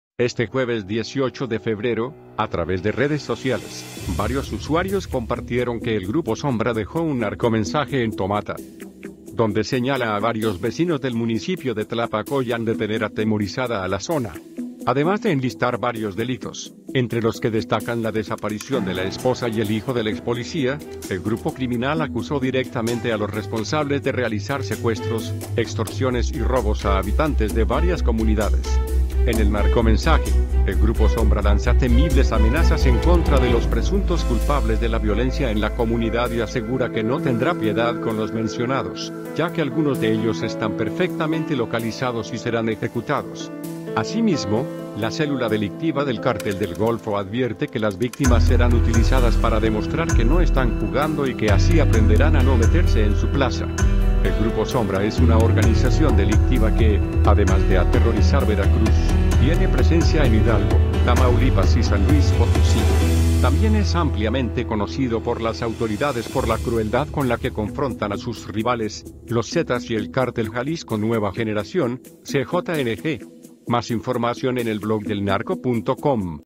Grupo Sombra del Cártel del Golfo estremece a Veracruz con narcomensaje
Donde señala a varios vecinos del municipio de Tlapacoyan de tener atemorizada a la zona.  Además de enlistar varios delitos, entre los que destacan la desaparición de la esposa y el hijo del expolicía, el grupo criminal acusó directamente a los responsables de realizar secuestros, extorsiones y robos a habitantes de varias comunidades.